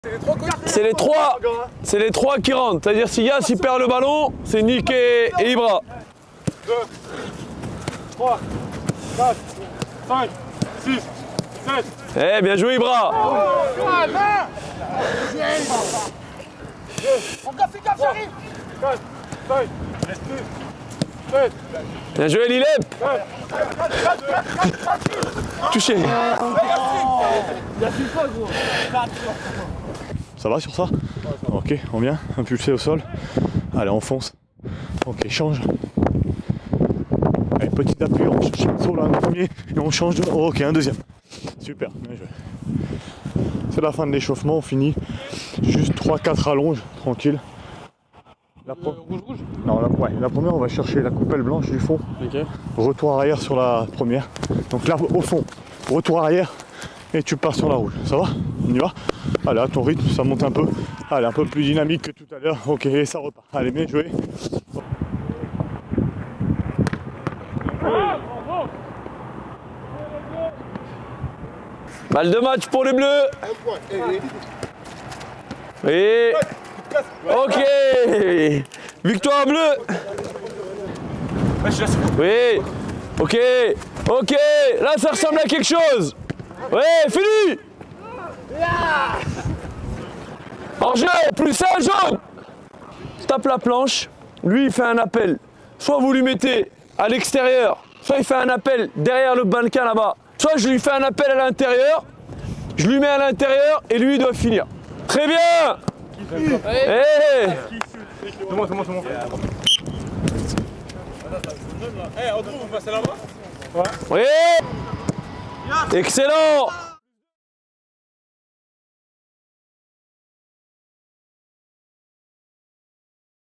Inside entraînement : au plus près des Valenciennois
Micro et camera activés, plongez au coeur de la séance au plus près du staff et des joueurs valenciennois.